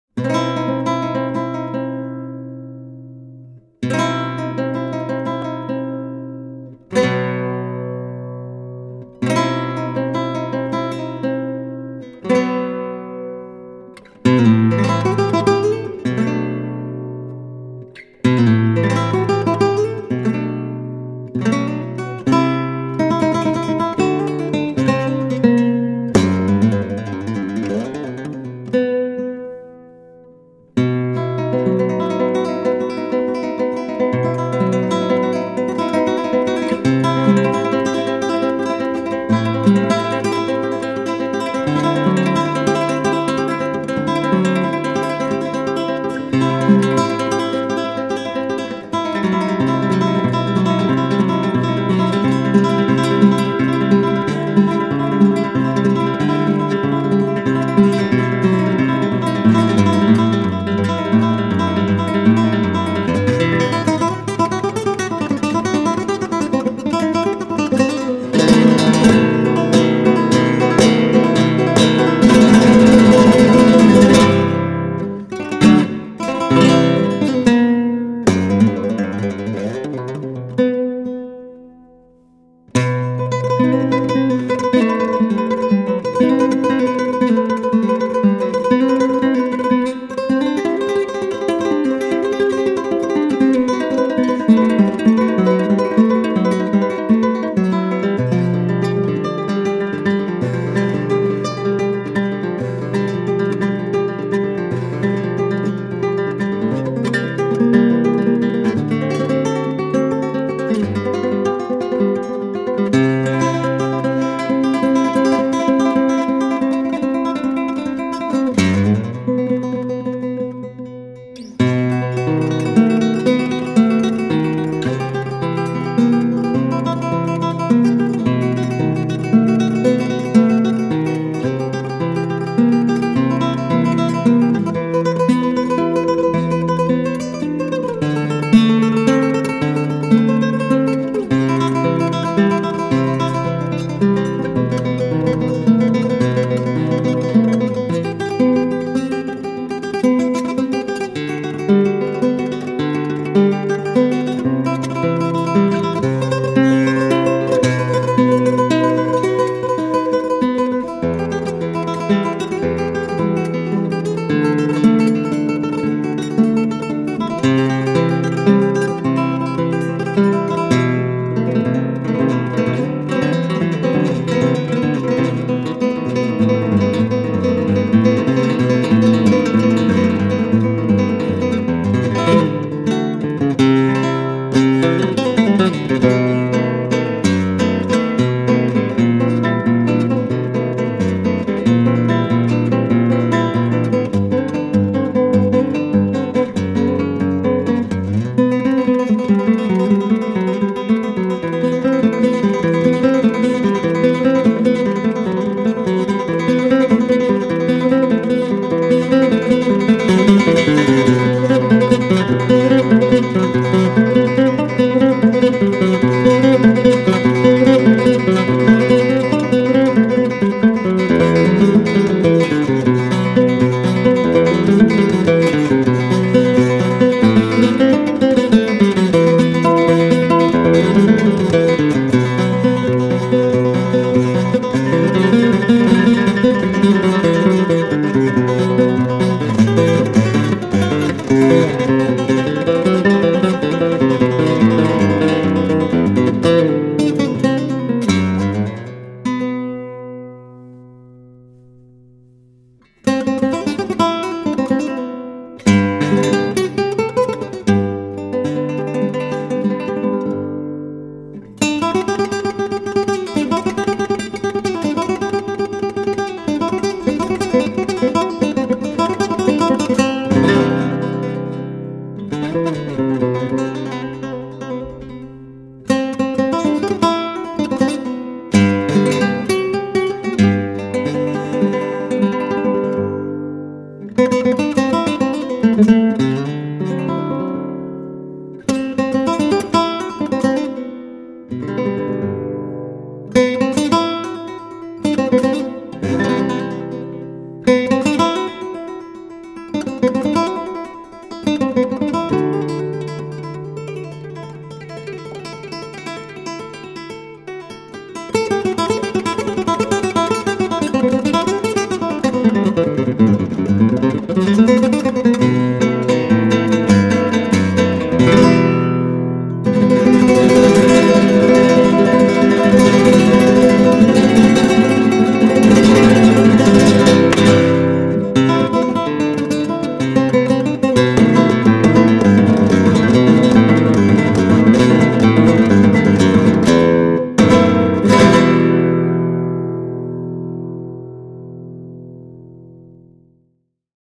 Final III Concurso Federación Peñas.
Final III Concurso Federación Peñas Flamencas Granada. Peña La Platería. 10 de enero de 2015